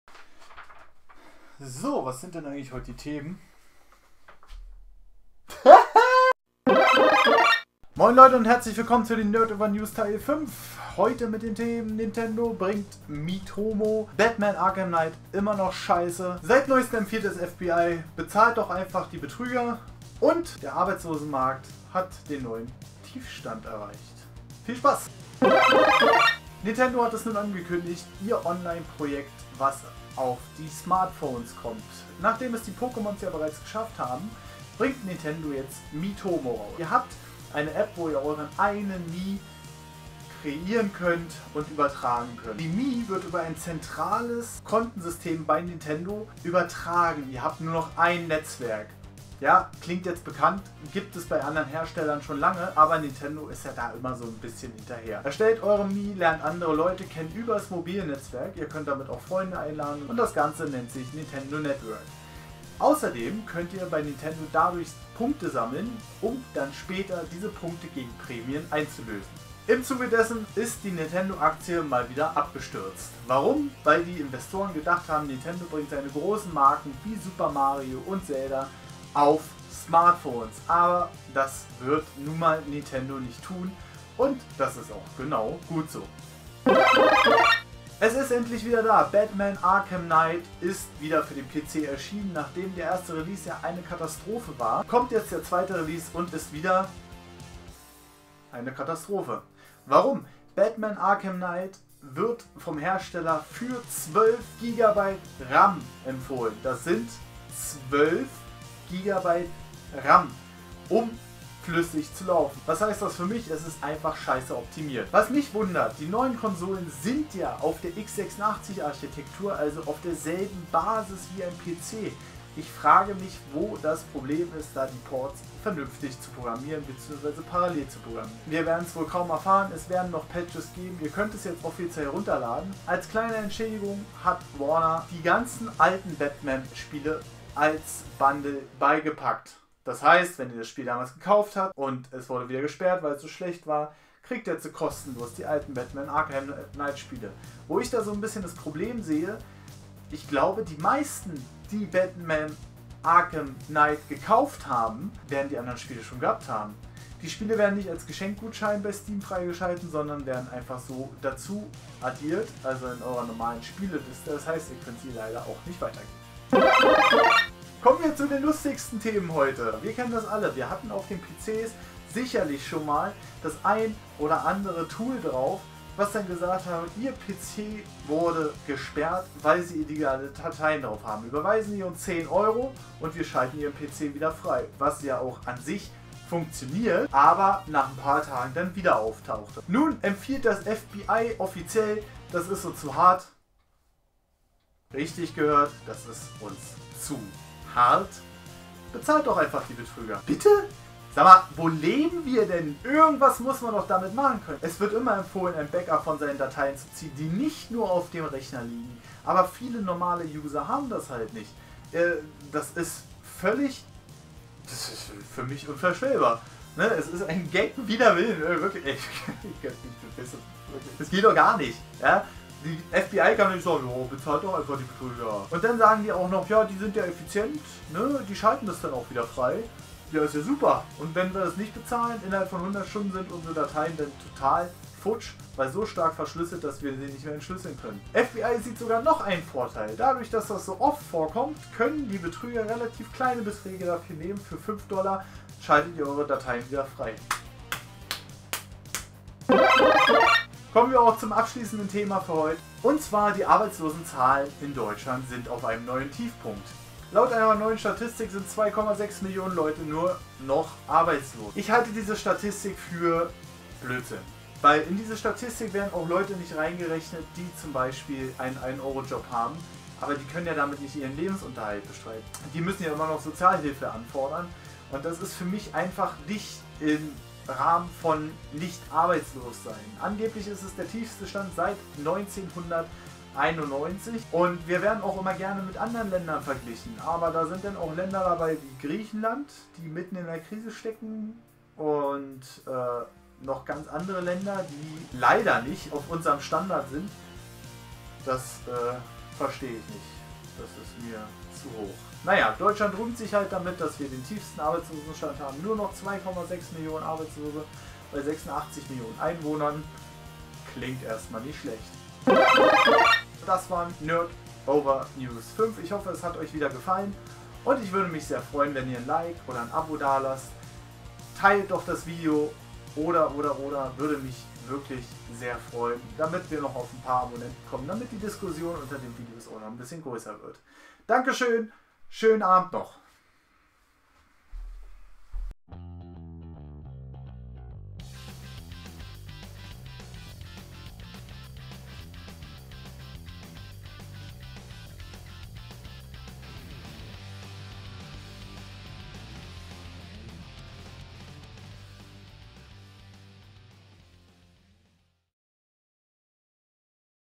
Nerd News #5 // FBI empfiehlt: Bezahlt Betrüger! // Miitomo // Batman PC
Nerd News #5 // FBI empfiehlt: Bezahlt Betrüger!// Nintendo Miitomo // Batman Arkham Knight immer noch kacke // Arbeitslosenzahlen auf "Tiefstand"